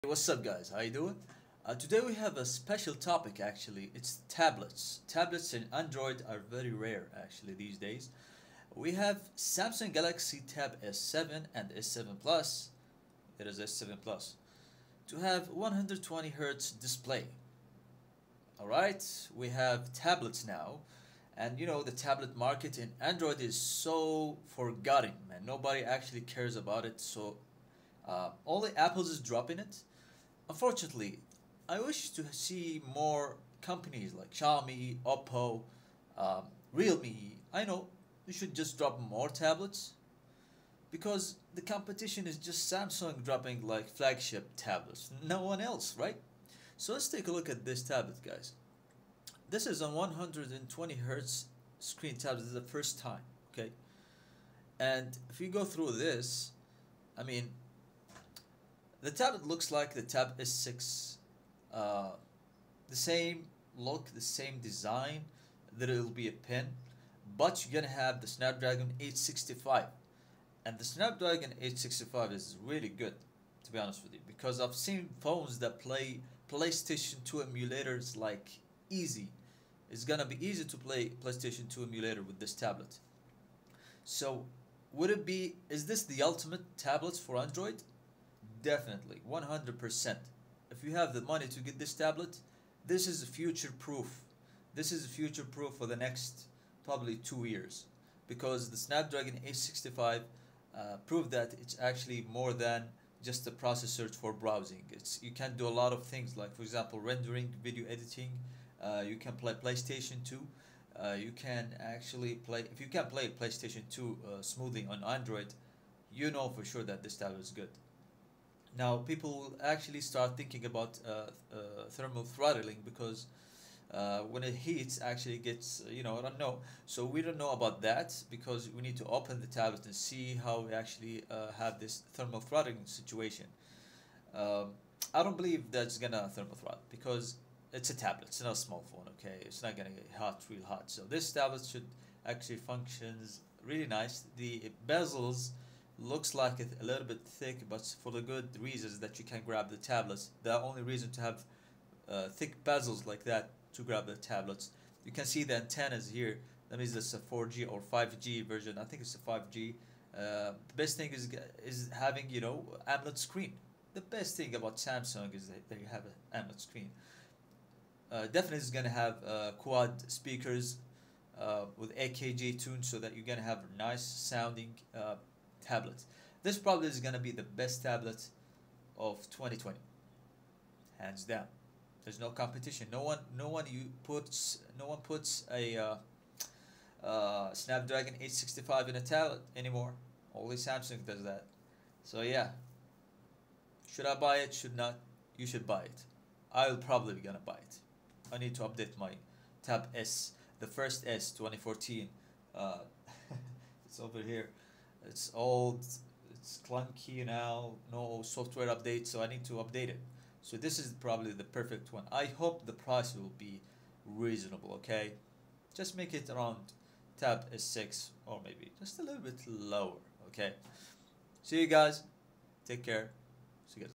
Tech News : Samsung Tab S7 is the best android tablet of 2020
Breakdown why this tablet will be the best and there's no competition to Samsung in tablet market.